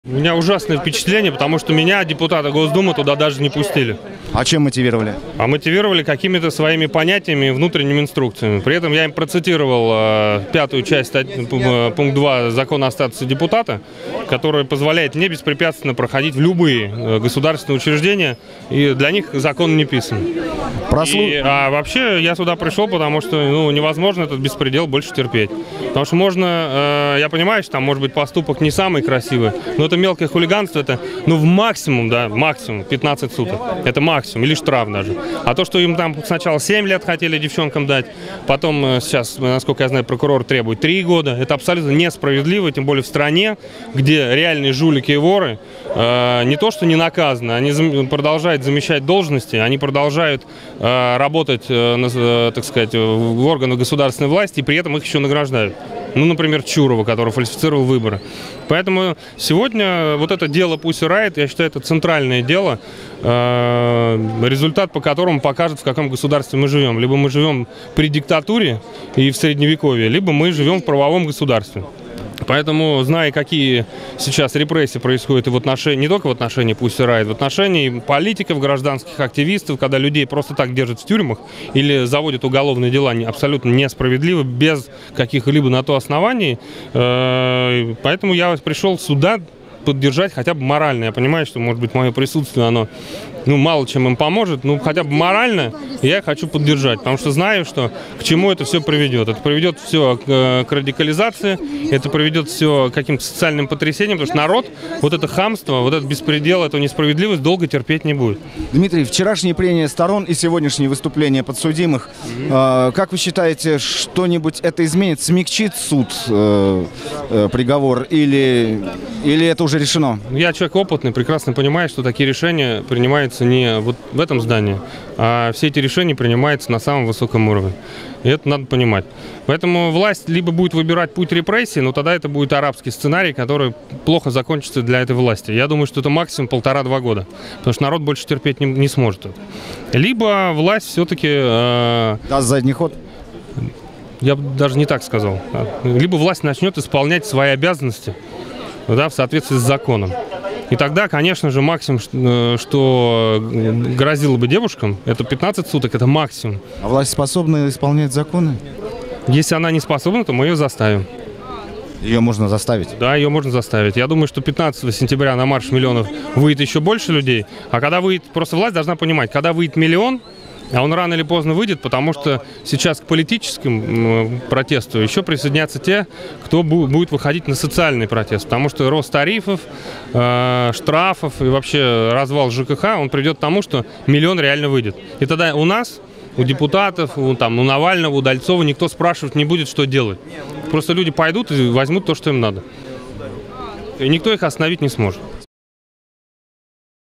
Дмитрий Гудков о суде над Pussy riot
Депутат ГД РФ Дмитрий Гудков - о своих впечатлениях от суда над Pussy riot и о развитии протеста в России. Смотрите интервью politvestnik.tv